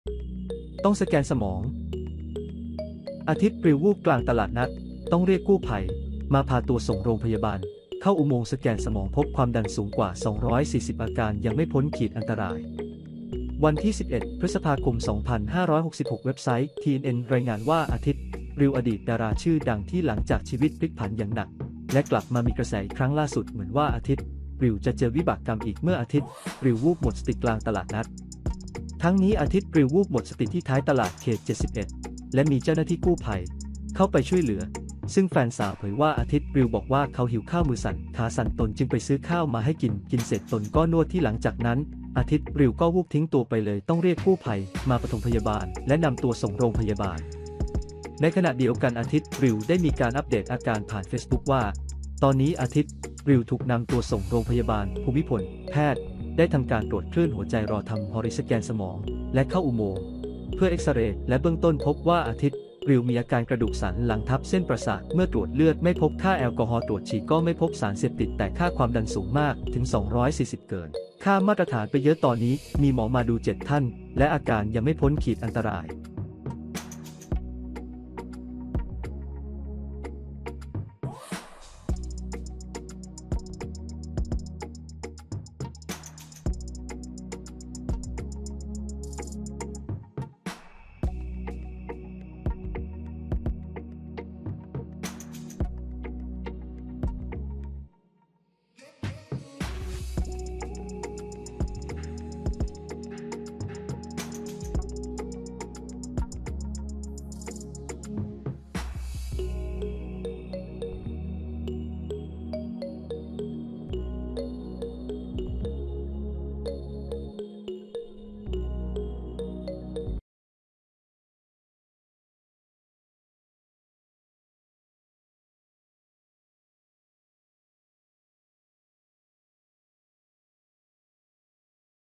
อัลบั้มภาพ อาทิตย์ ริว วูบกลางตลาดนัด ความดันขึ้นสูง 240 ต้องสแกนสมอง ยังไม่พ้นขีดอันตราย
อัลบั้มภาพ เบื้องต้นไม่พบแอลกอฮอล์